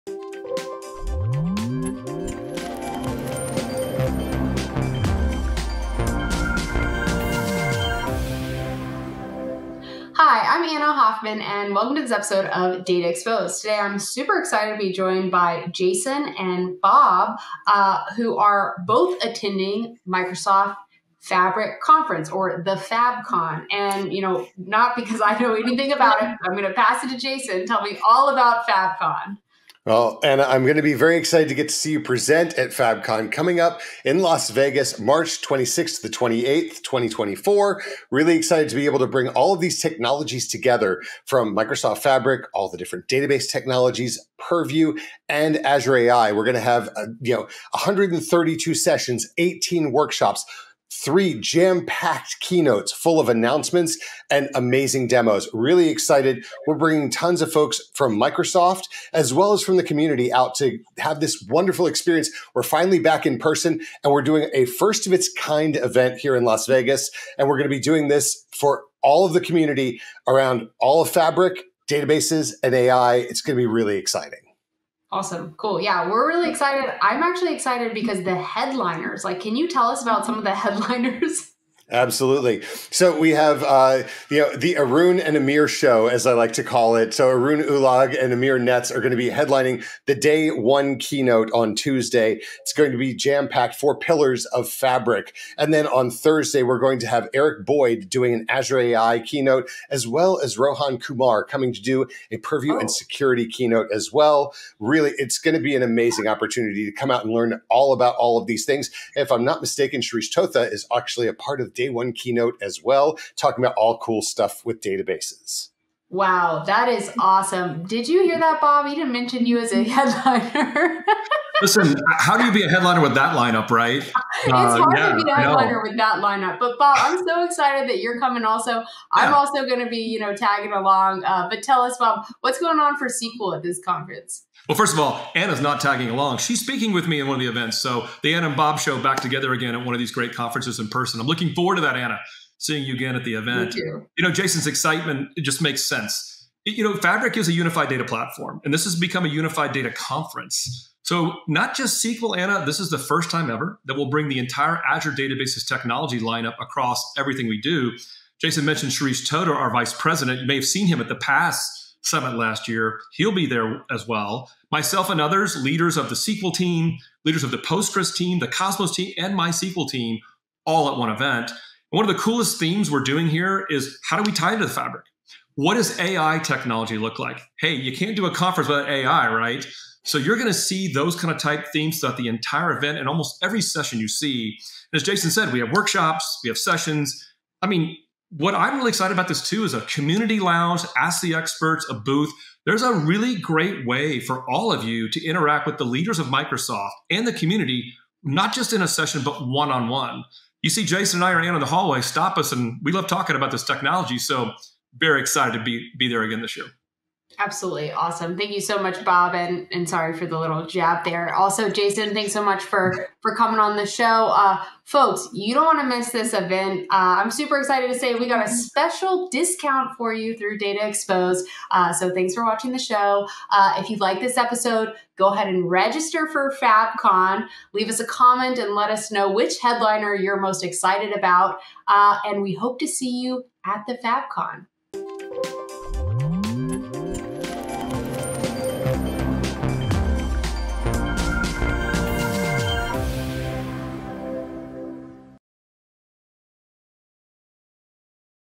Azure Data @ Microsoft Fabric Community Conference 2024 | Data Exposed Exclusive
In this Data Exposed Exclusive, join Anna Hoffman, Bob Ward, and Jason Himmelstein as they discuss everything you need to know about the upcoming Microsoft Fabric Community Conference!